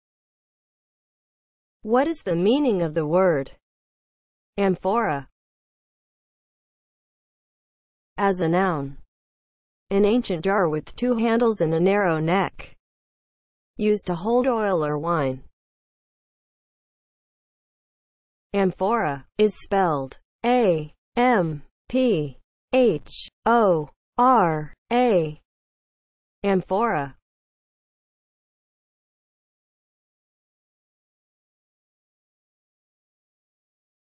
What is the meaning of the word AMPHORA?
Definition and spelling of the word AMPHORA.

(00:00) Intro
(00:07) As a noun
(00:17) Spelling

As a noun (00:07):
• an ancient jar with two handles and a narrow neck; used to hold oil or wine